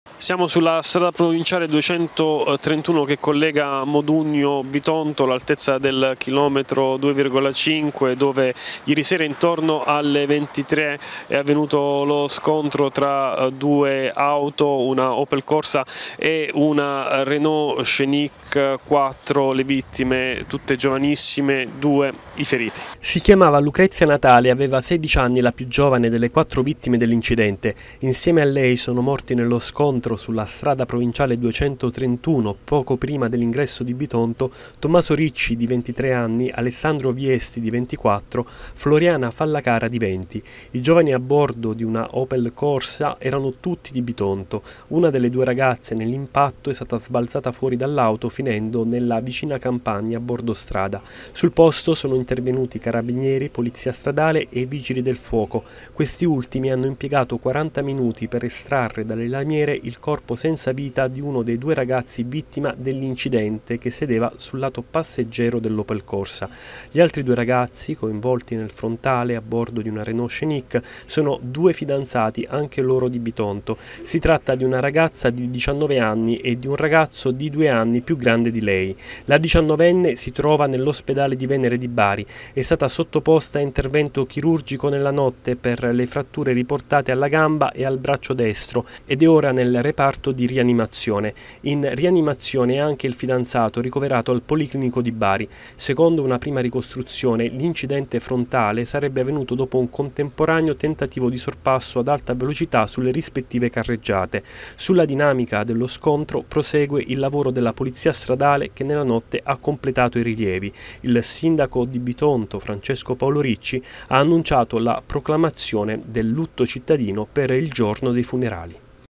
Tragedia tra Modugno e Bitonto, scontro frontale tra due auto: morti quattro giovani, due feriti
Le vittime, tutte a bordo di una Opel Corsa, avevano tra i 16 e i 24 anni